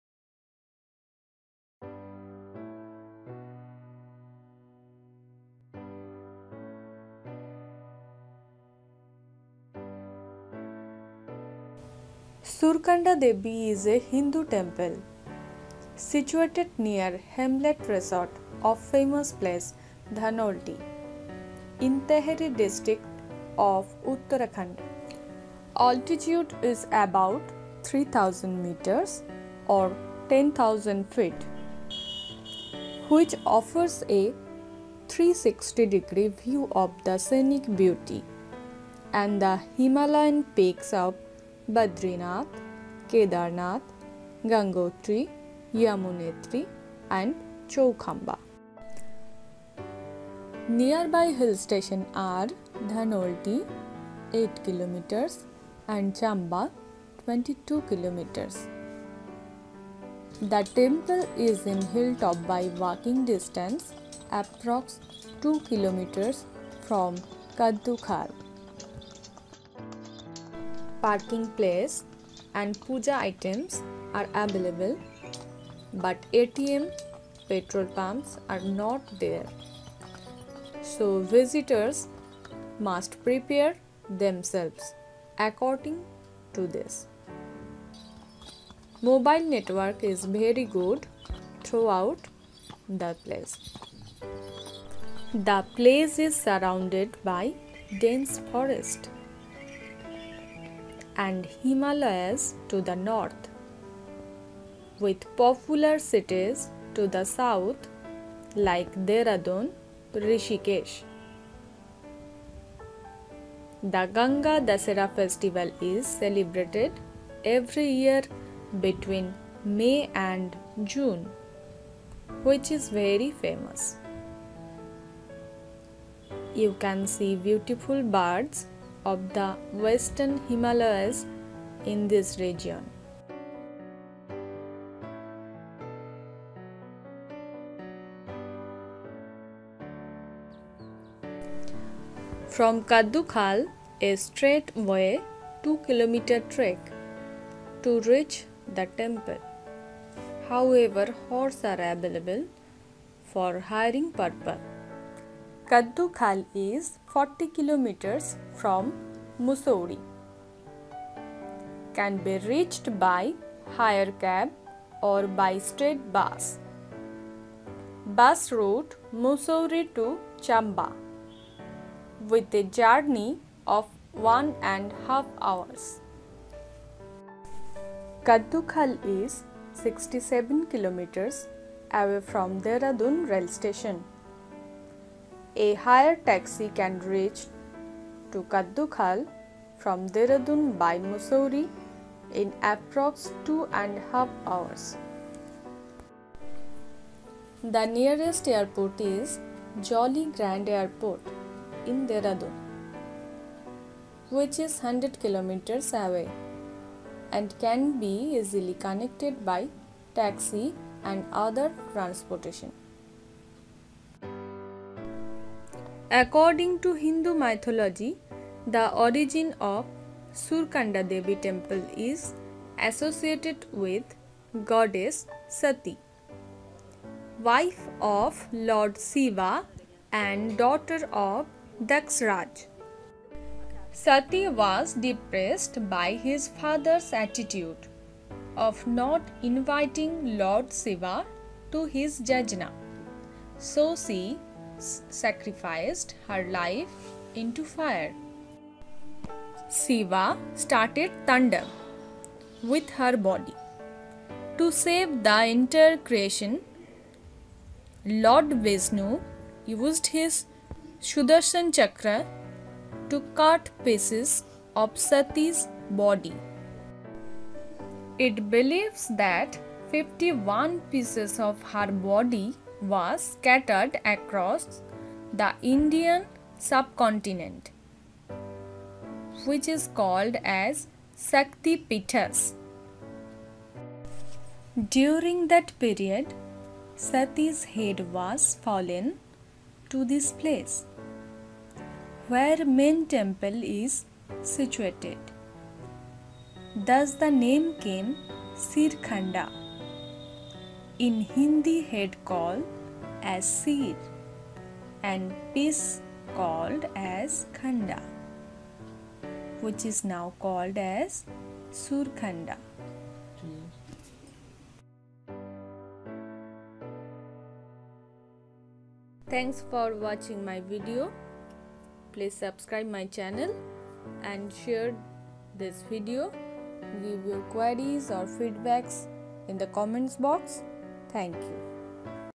Full Story of Surkanda Devi Temple Dehradun Mussourie Dhanoulti Uttarakhand
Complete story of Surkhanda or Shirkhunda devi temple at Dehradun Distrirct of Uttarakhand state of India.